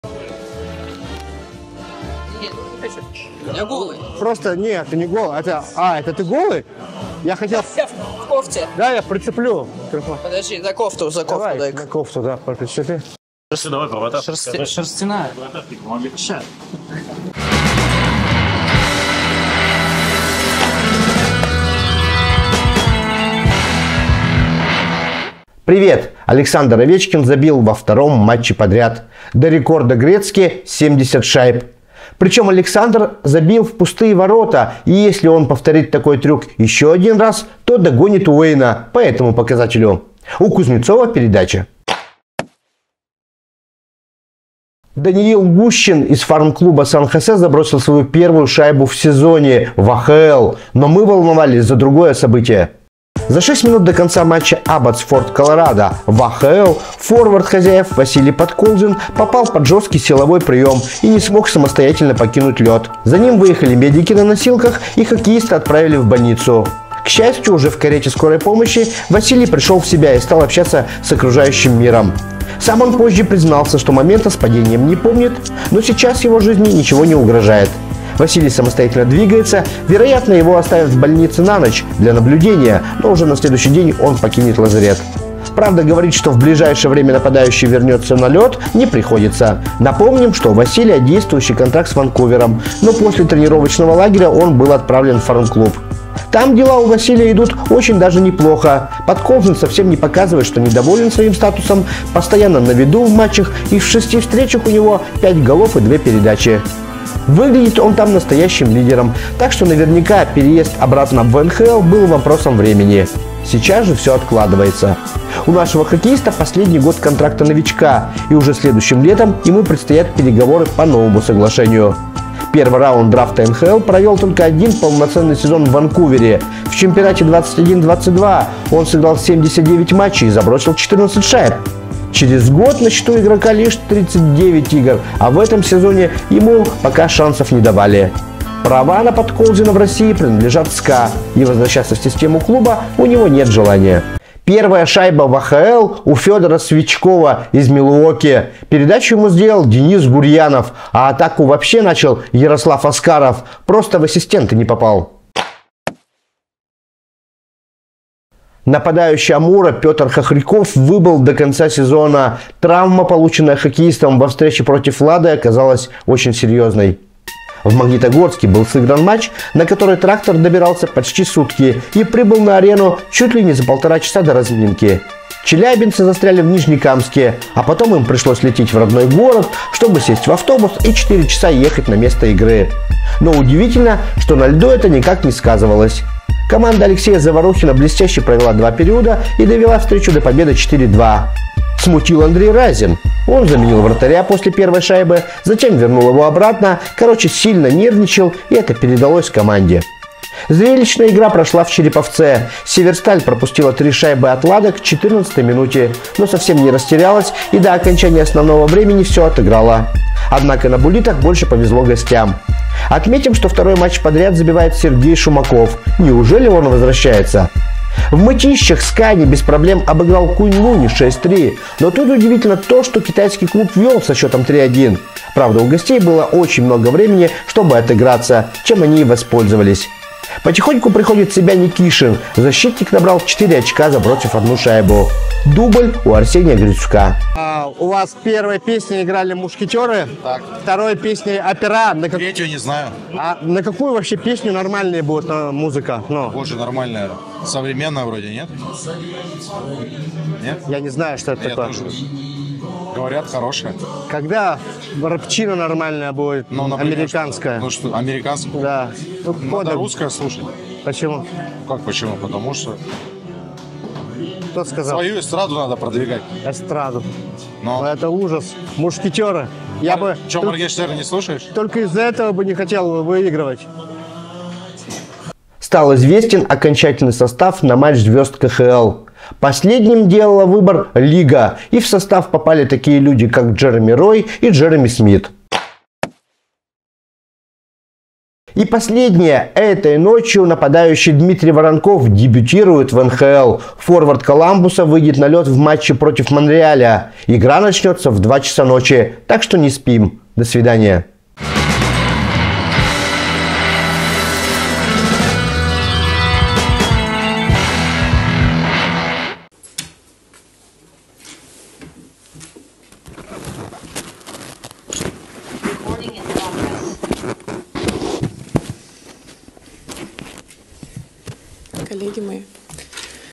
Василий Подколзин попал в больницу после матча в АХЛ. День с Алексеем Шевченко
Овечкин скоро догонит Гретцки, "Металлург" проиграл "Трактору", а Дмитрий Воронков скоро выйдет на лед в НХЛ.